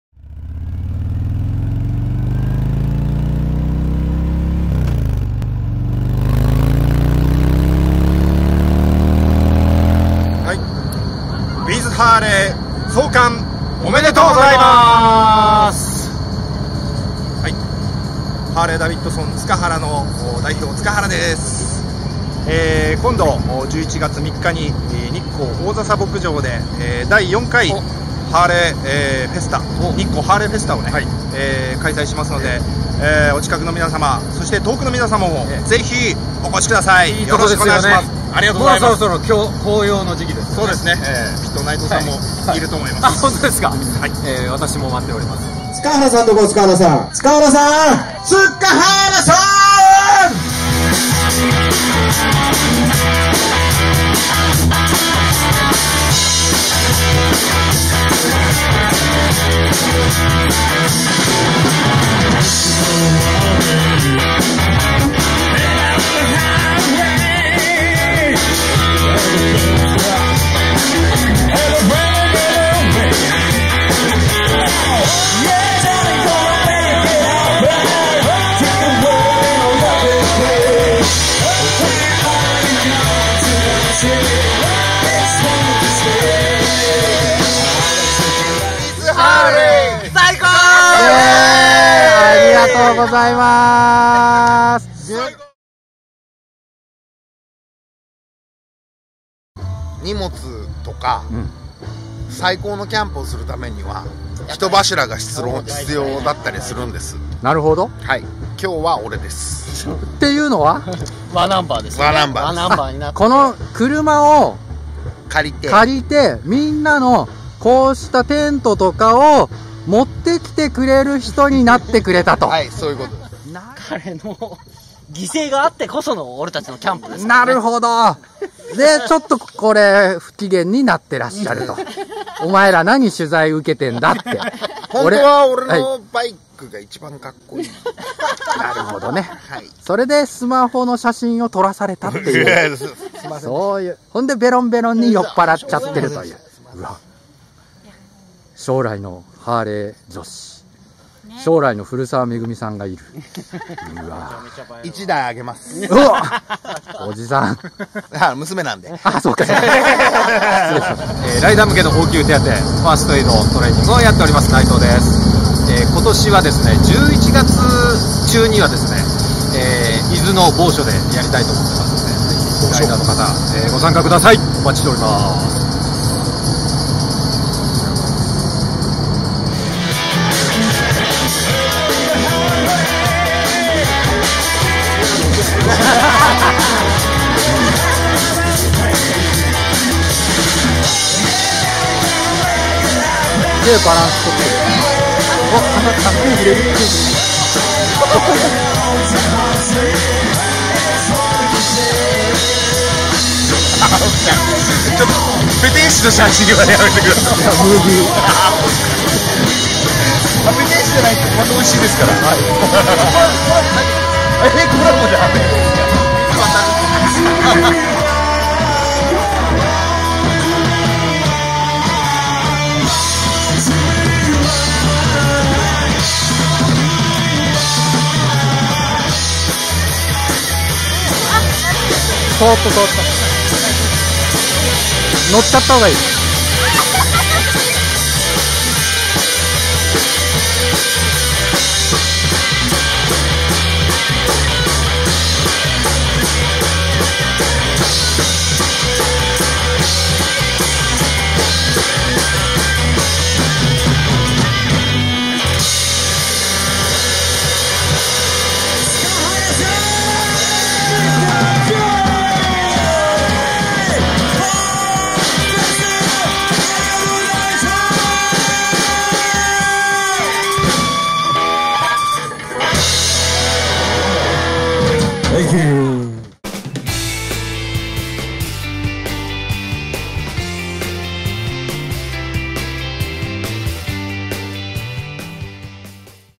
バイク、アウトドア、音楽が融合「BOMフェス！ 浜通り」レポート
日本のライダー有志が手掛けるキャンプフェス 「BOM！！！ 浜通り」は、バイク、アウトドア、音楽が融合したイベントとして、震災からの復興が進む浜通りの街並みと太平洋を望む最高のロケーションの天神岬スポーツ公園にて2019年9月21日(土)・22日(日)、開催されました。

SPECIAL THANKS
CVO BAND
伊勢悟
内藤学
H-D塚原